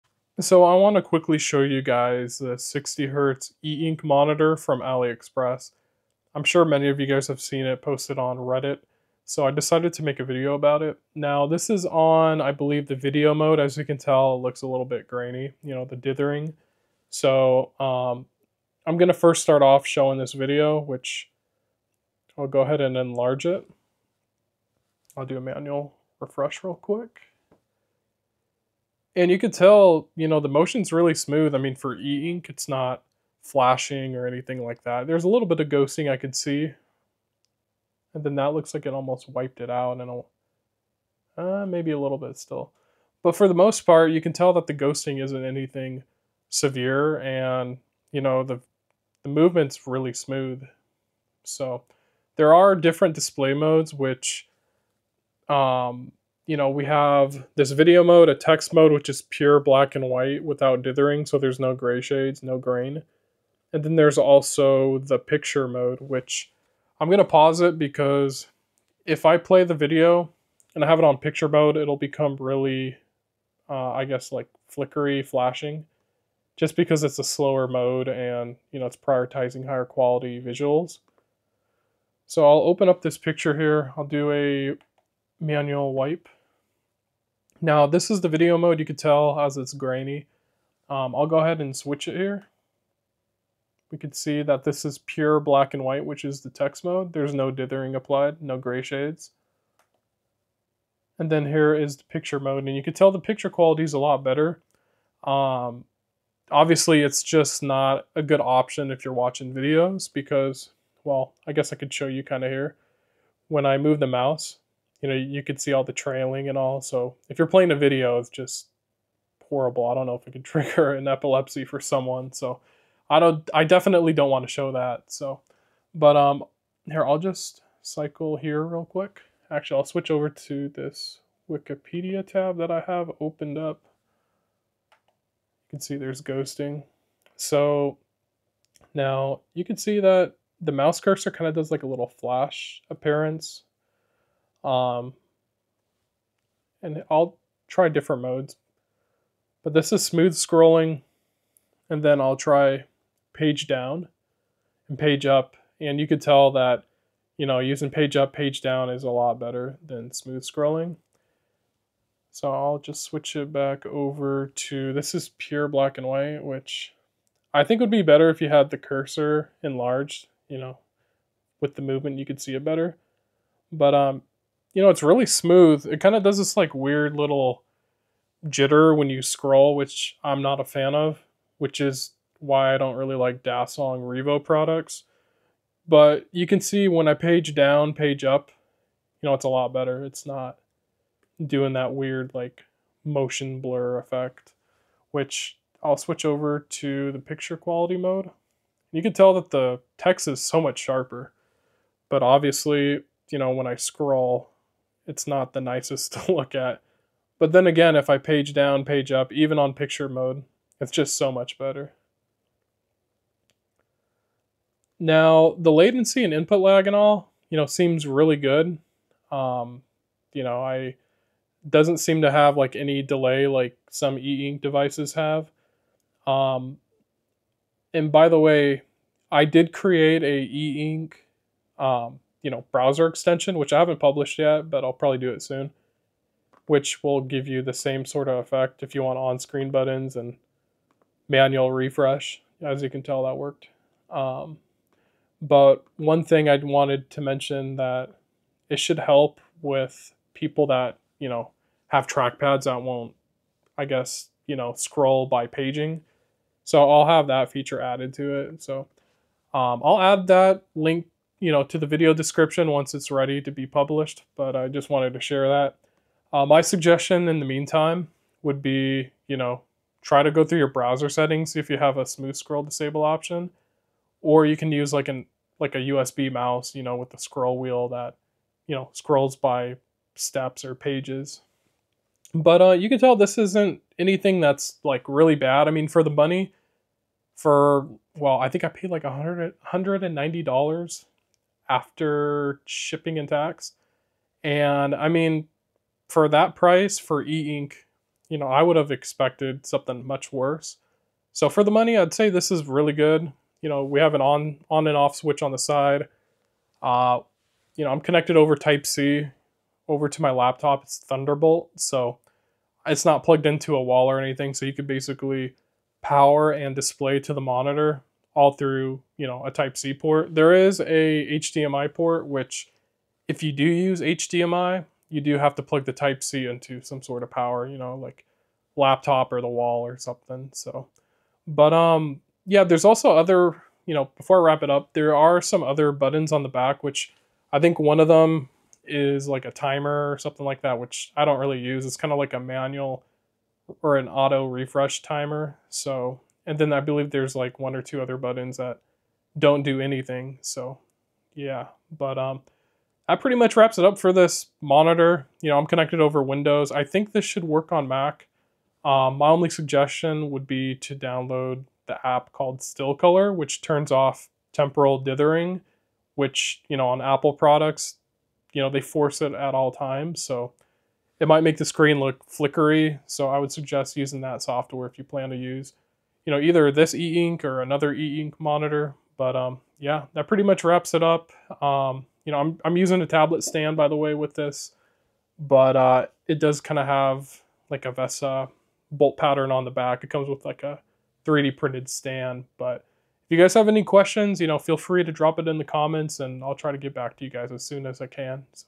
60Hz E Ink Monitor for Under $200 – Is It Worth It?
Today I am going over the cheap 60hz eink monitor from aliexpress. This monitor is really eye friendly but does have a really glossy screen if thats an issue to some. It is bright white and not a dark panel.